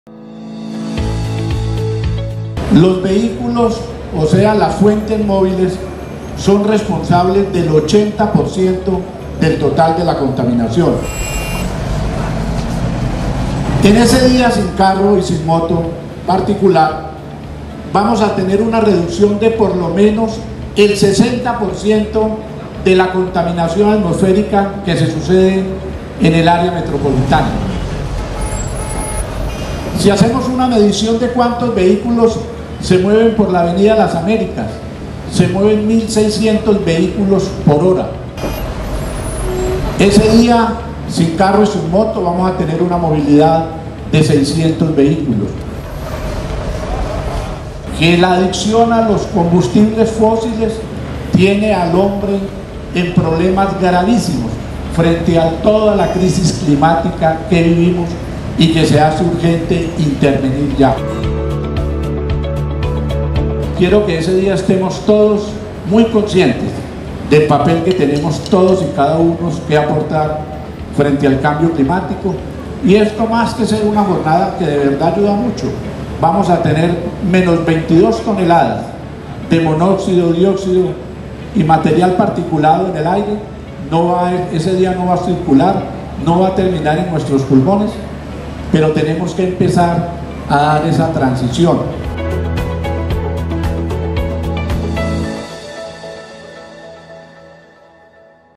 EL CAMBIO CLIMÁTICO AVANZA, CUIDEMOS EL MEDIO AMBIENTE
Carder MedioAmbiente Risaralda SiembraFuturo Este 27 de septiembre de 2022 tendremos el ❌🛵🚗‘Día sin Carro y sin Moto’ en el Área Metropolitana y Santa Rosa de Cabal. 🌳Usemos transportes sostenibles, alternativos y solidarios.